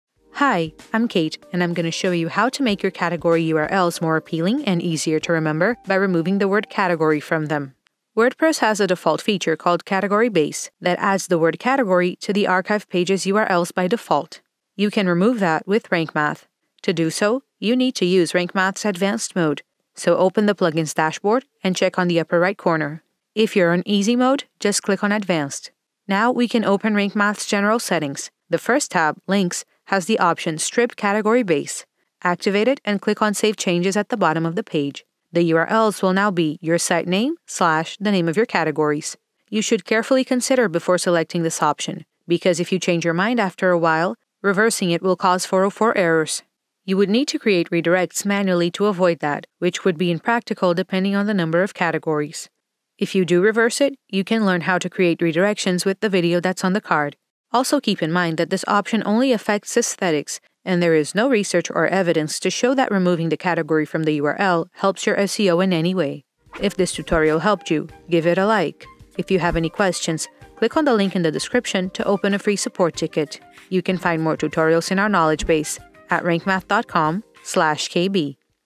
Remove the Category Base From WordPress URLs - Rank Math SEO
WordPress includes a default URL base for your category pages by default. But you can always remove them to keep your URLs short.

Rank Math makes it easy to remove your category base at the click of a button without writing any rewrite rules. This video will show you how you can remove category base from your WordPress site using Rank Math.